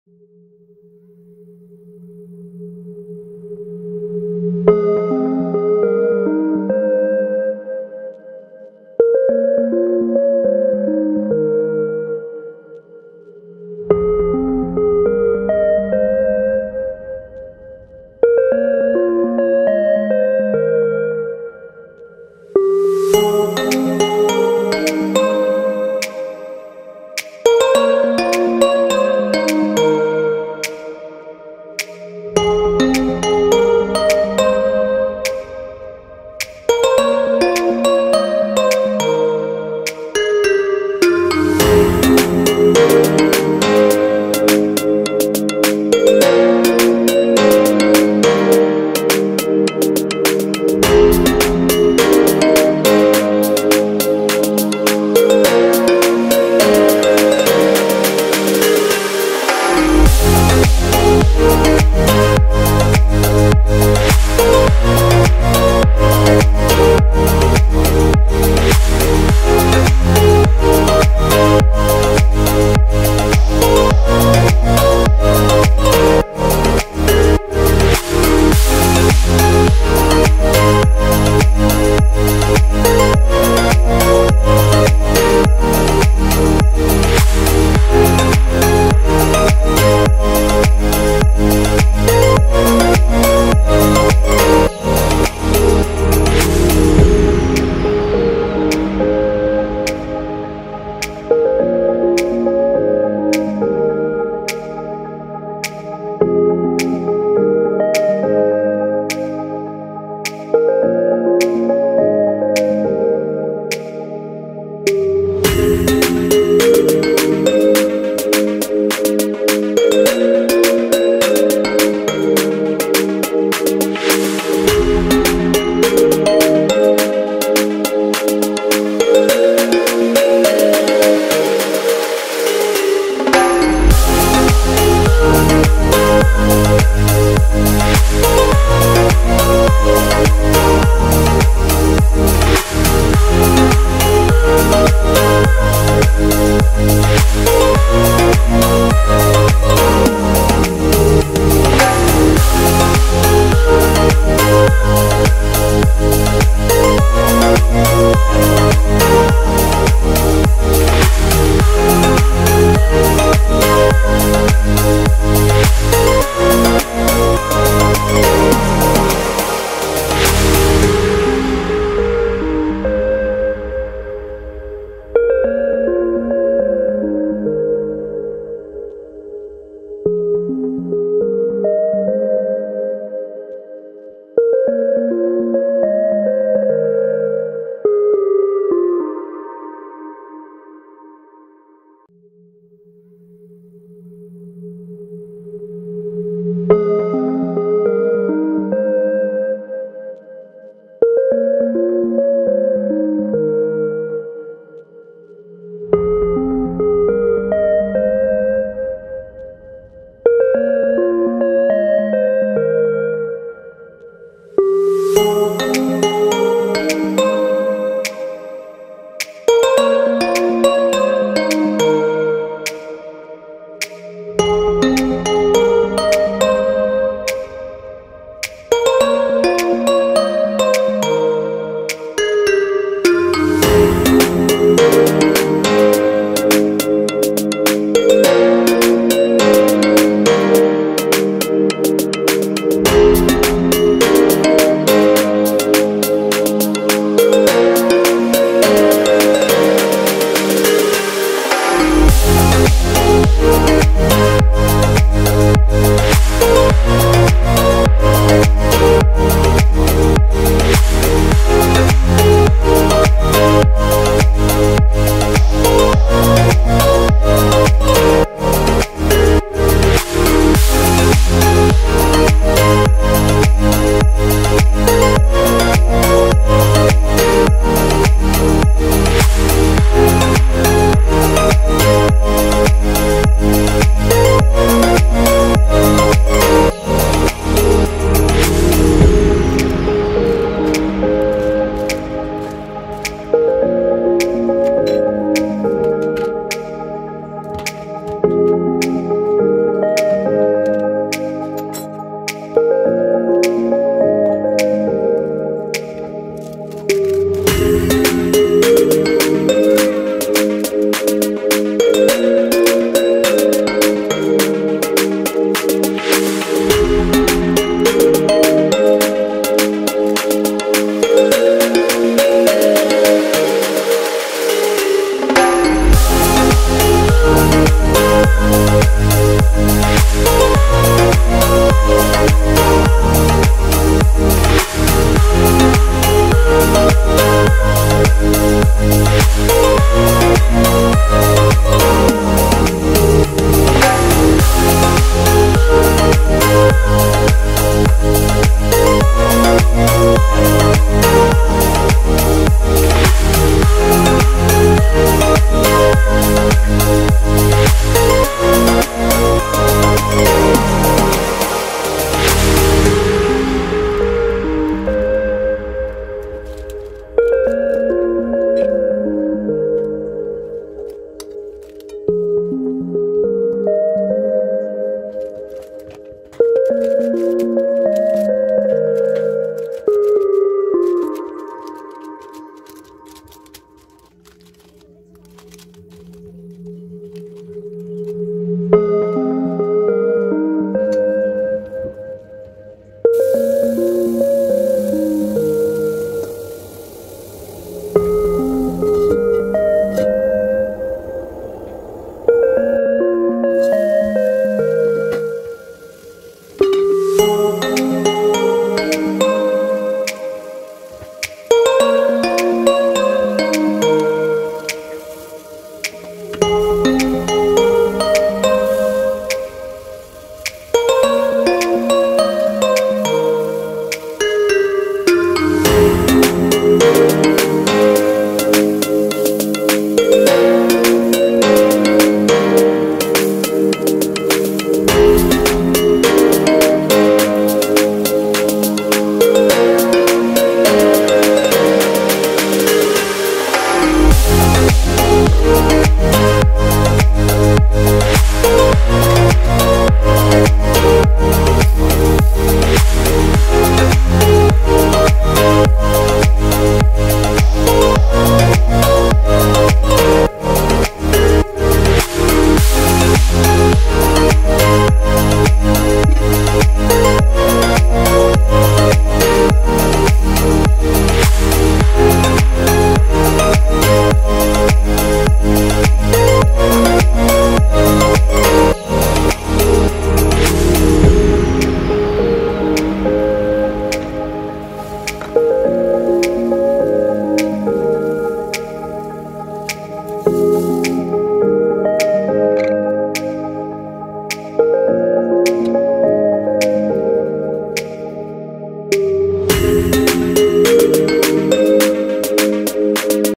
My Sweet, Spicy and Garlicky Skinless Longanisa
Skinless longganisa refers to a type of Filipino sausage without the casing. It is also known as naked sausage. Filipino Longganisa can either be sweet or garlicky. This version has the best of both worlds. It also has no preservatives. This is best when eaten with sinangag na kanin along with itlog na maalat or salted egg and papaya atchara. I like to dip it in spicy vinegar before eating. Yummy!
Cooked in saladmaster without oil.

Recipe:
1 kg ground pork
1 1/2 tsp salt
1 tsp ground pepper
1 1/2 ground garlic head
1 tbsp oyster sauce
3 tsp cornstarch
1 1/2 tsp sweet paprika(optional)
Chili pepper
Combine all and leave in the fridge for about a few hours.

Happy cooking!

Musician: @iksonmusic